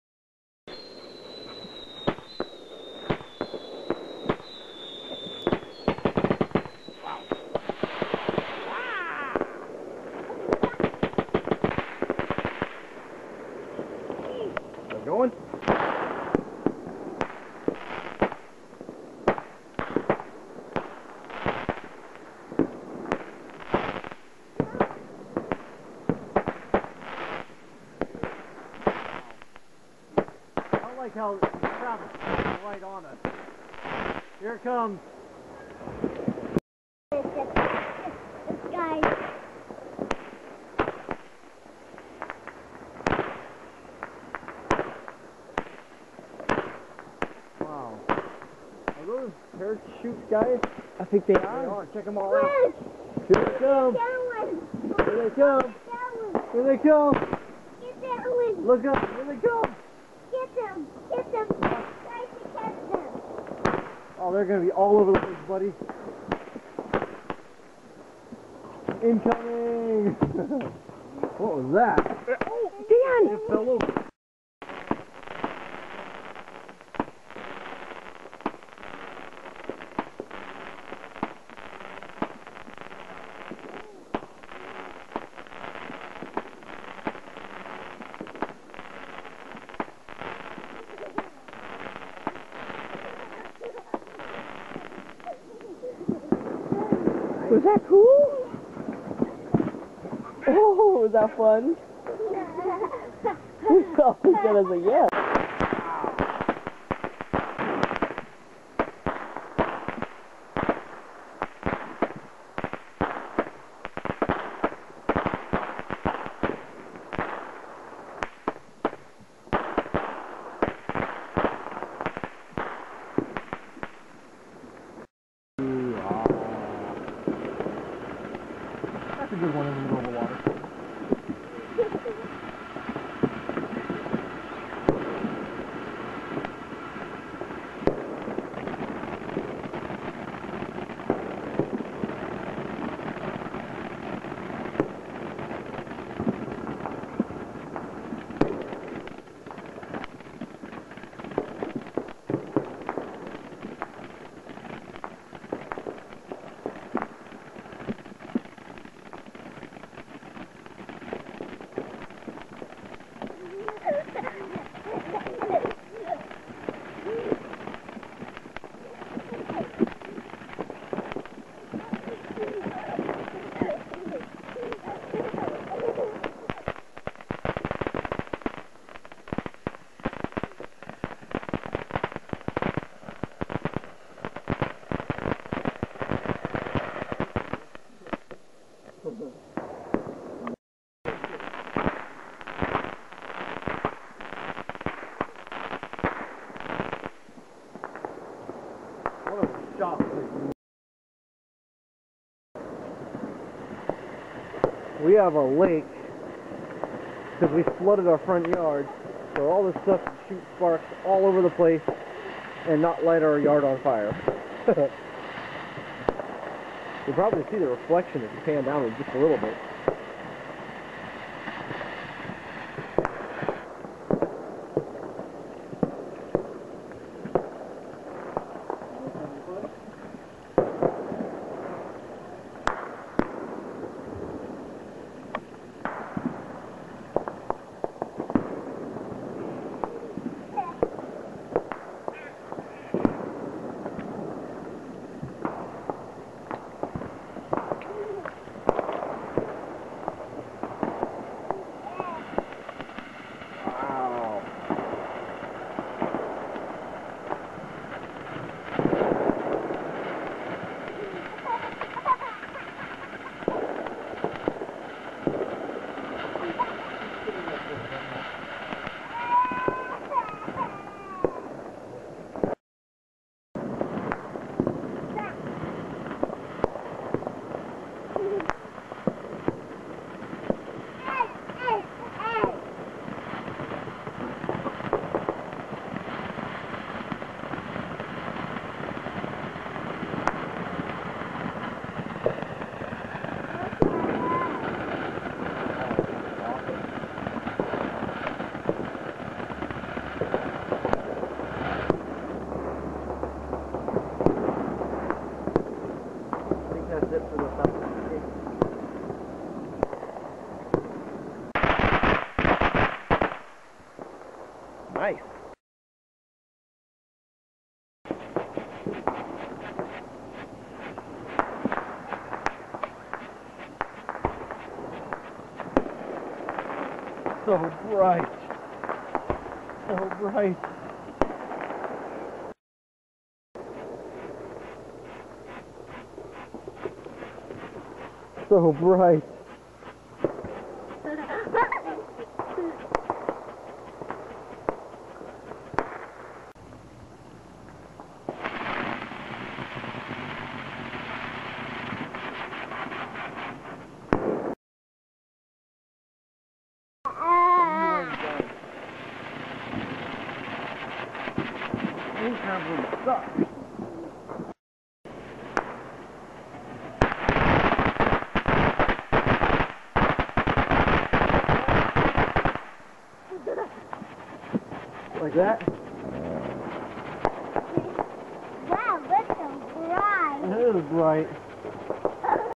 4th of July 2009
Some of  the 4th of July fireworks display at the Graff House on 4 July 2009